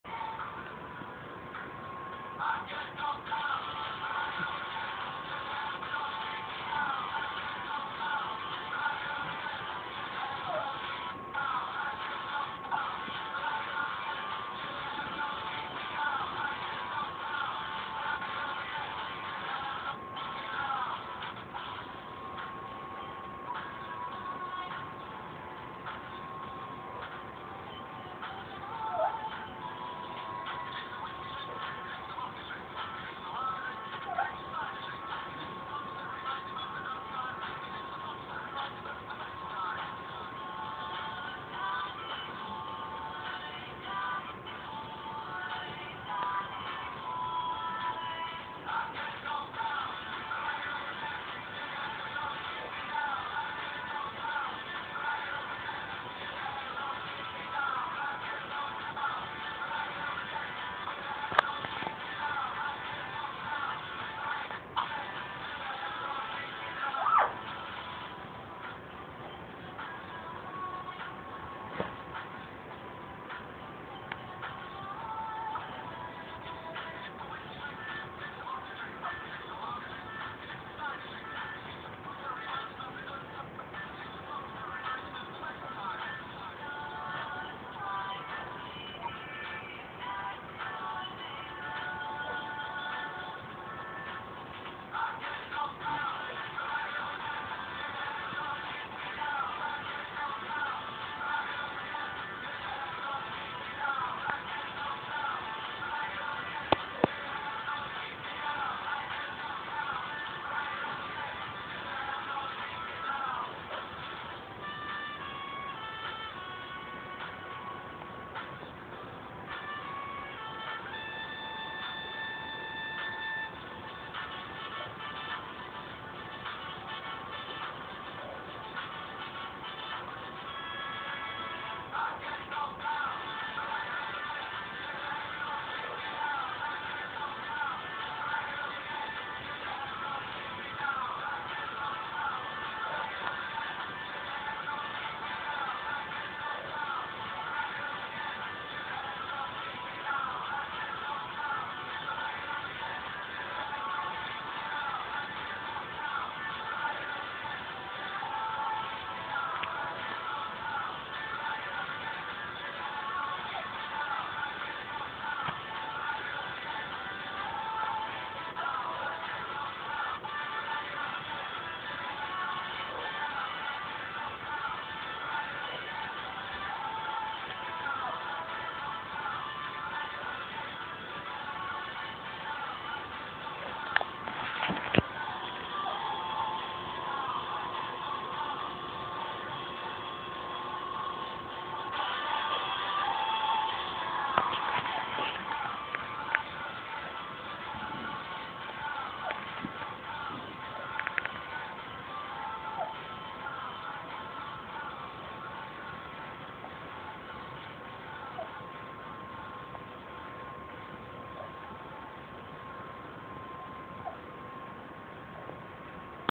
naylor rd knock down
2 house nocked down in naylor rd birkenhead north.